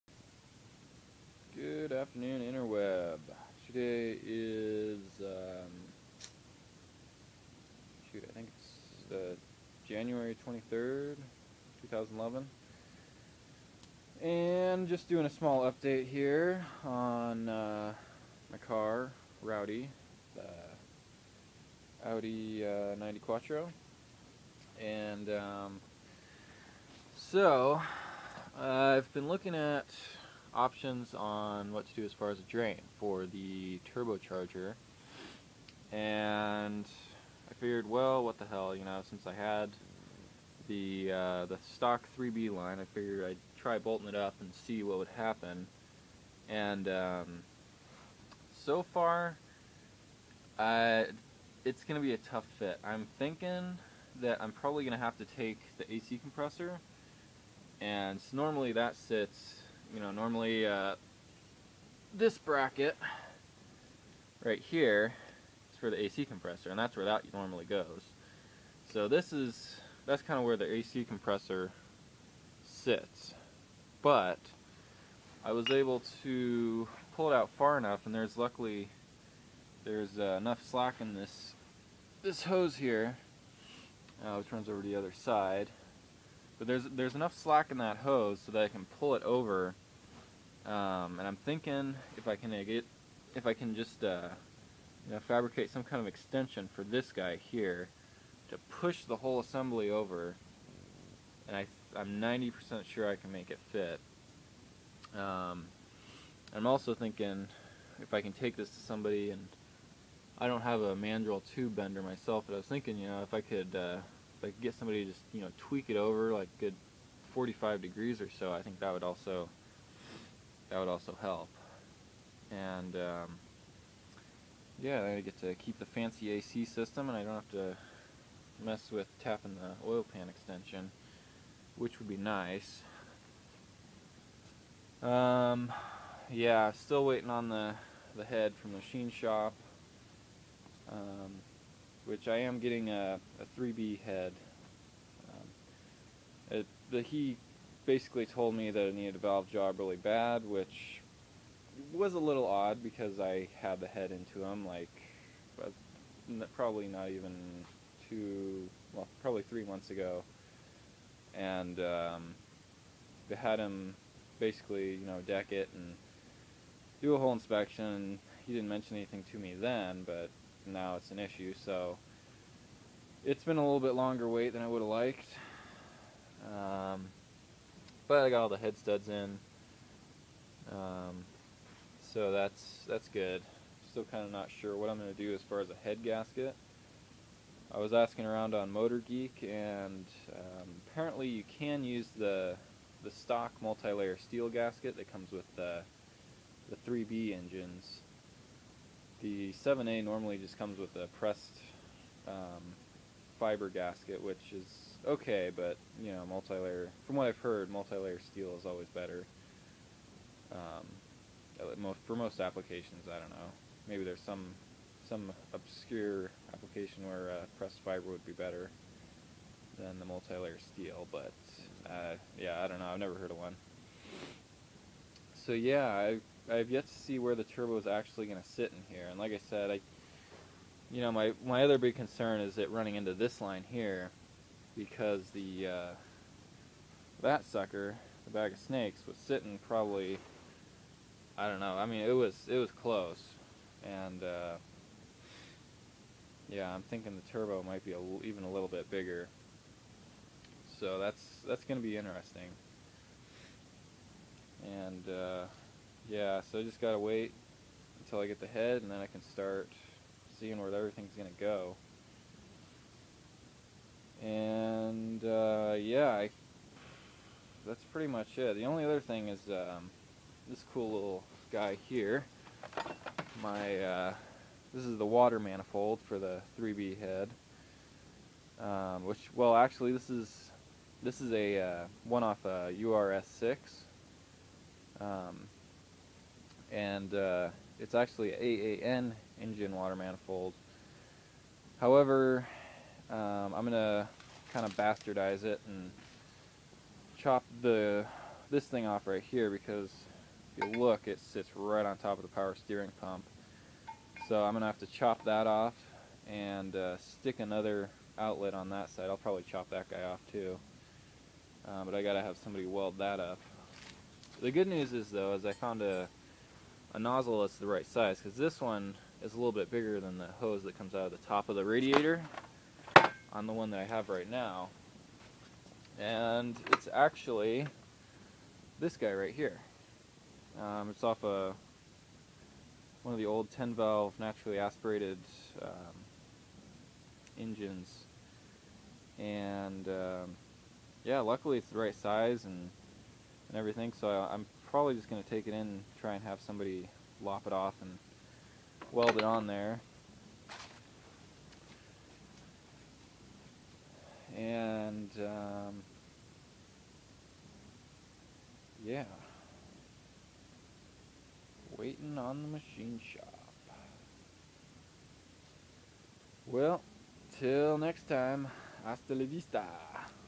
Waiting on the machine shop.......
A short update, mainly just talking about turbo lines and head gakets. and this was filmed in 2013, I don't think I had quite enough ventilation going when I was degreasing the engine. :(